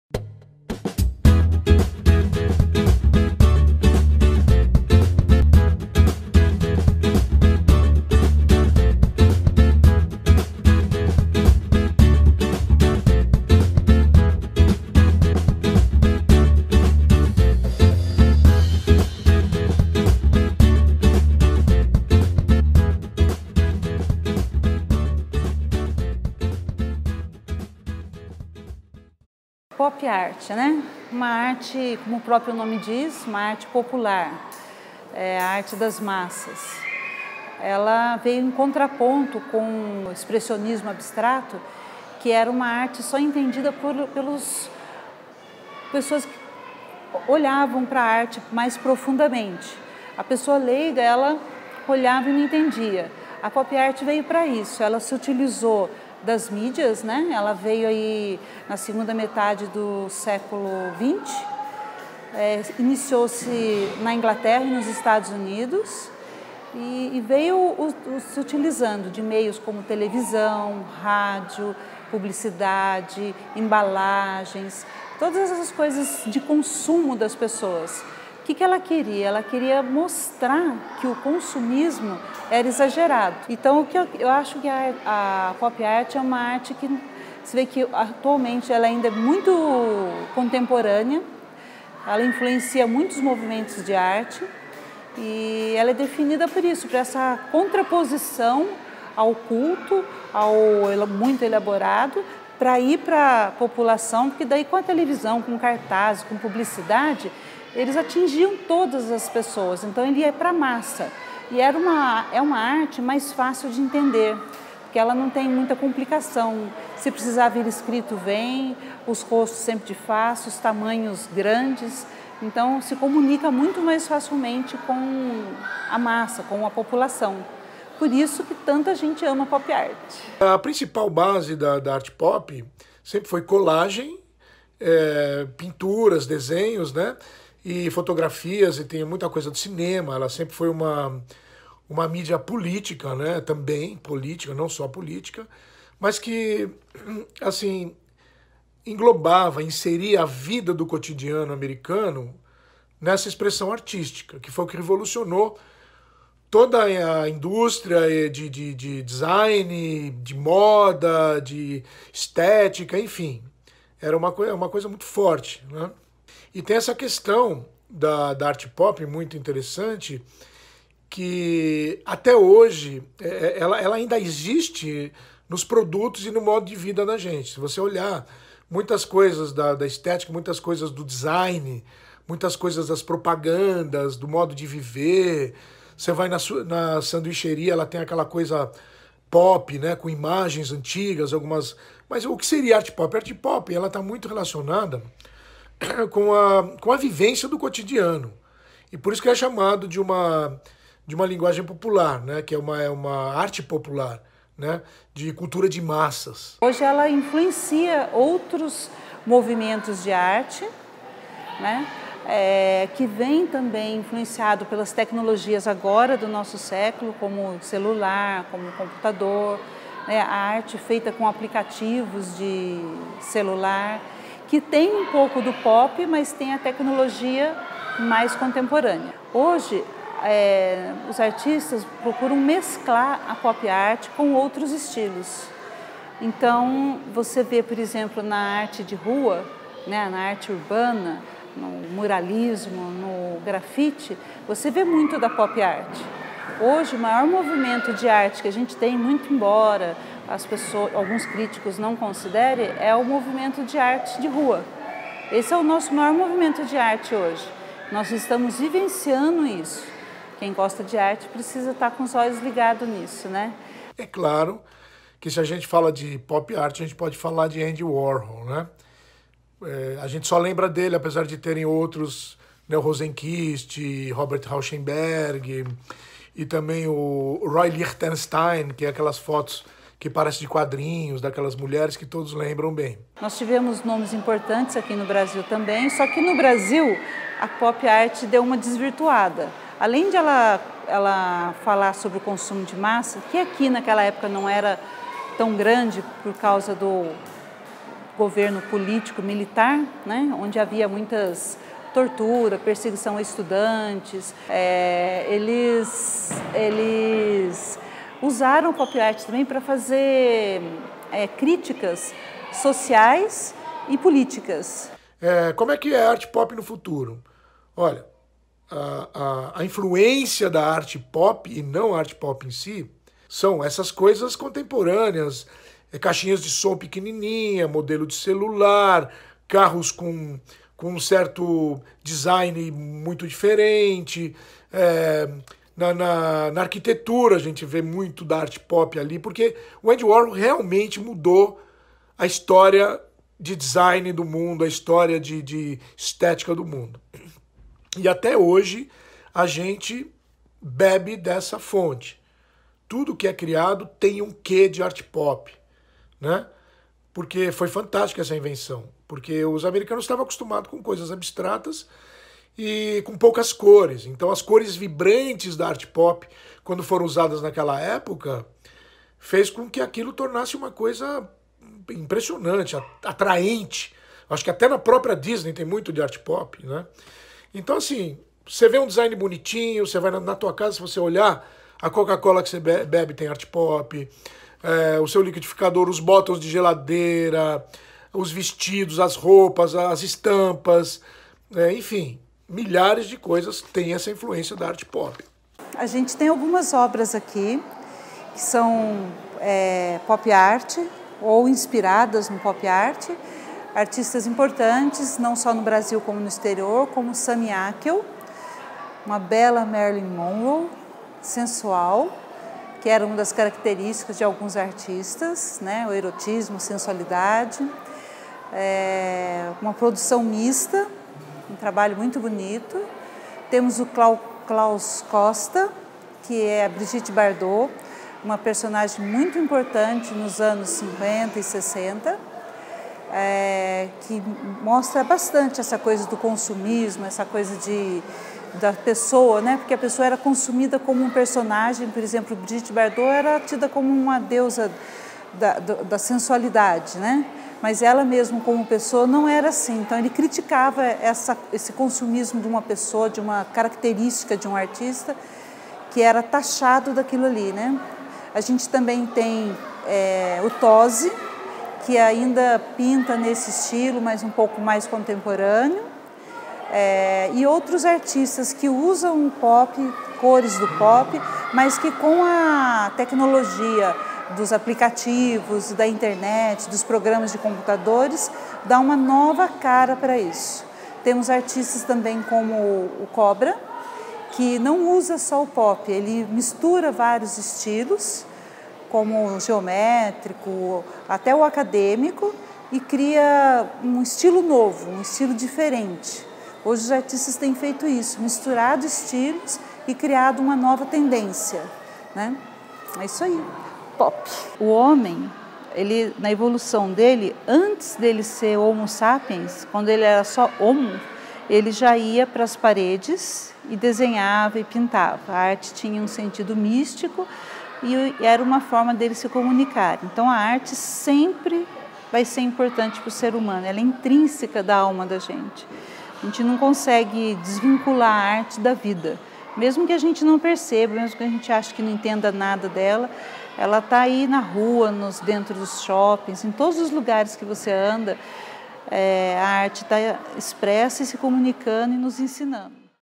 POP ART E ATUALIDADE
Com o desejo de aprofundar o trabalho sobre Pop Art solicitado na disciplina de Estética e História da Arte, as “jornalistas in-formação”, Valeska Migotto (Blog @ReinaçõesdeJornalista), Fernanda Paiva e Juliana Faria (Blog @JuFaria) produziram esse “minidoc” com a colaboração da Galeria Victor Hugo de São José dos Campos e dos artistas plásticos Cristina Demétrio e Sami Akl, que nos contaram um pouquinho mais sobre o que é Pop Art e suas influências nas expressões artísticas atuais.  Uma troca de conhecimento enriquecedora e que gostaríamos de compartilhar com vocês. Quer entender um pouco mais sobre a Pop Art? É só dar play aqui em baixo vídeo 
Nossos sinceros agradecimentos aos colaboradores desse trabalho pela dedicação, pontualidade e carinho! Aos nossos amigos do seminário apresentado em sala, muito obrigada!